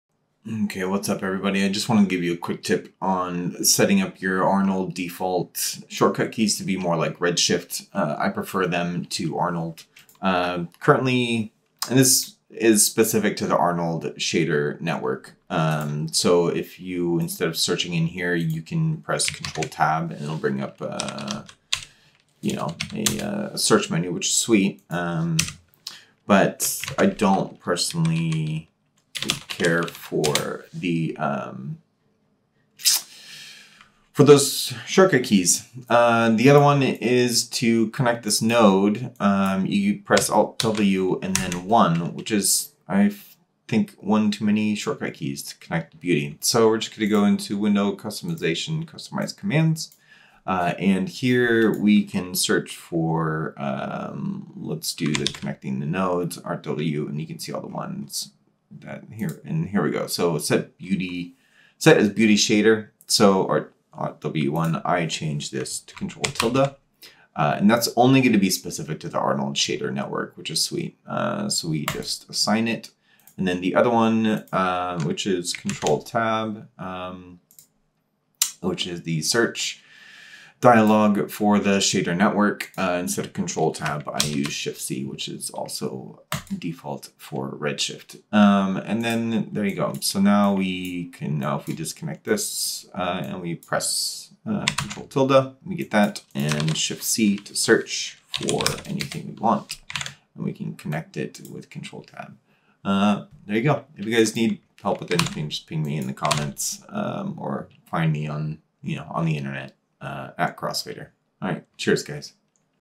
Cinema4D Arnold to Redshift Shader Network Shortcut Keys
If you didn't know the shortcut keys to Arnold in C4D or just switched and love the Redshift shortcut keys better, here's a quick tip on how to change them.